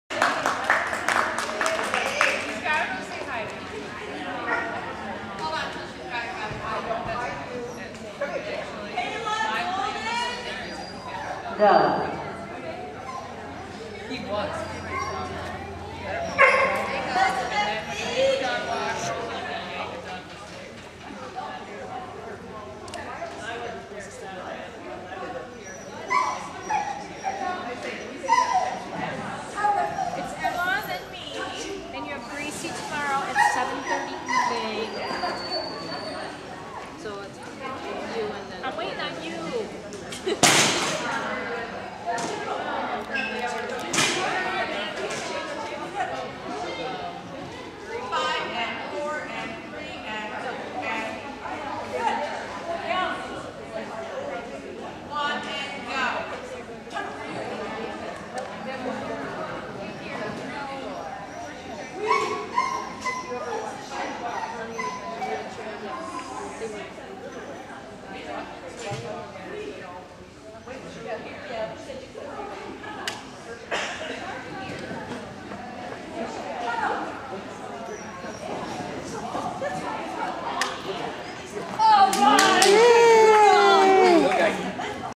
03-19-2010 Callie Novice Standard
Anne and Callie, Novice Standard Oakland County Kennel Club Trial - Canine Sports Recreation Center in Dexter, Michigan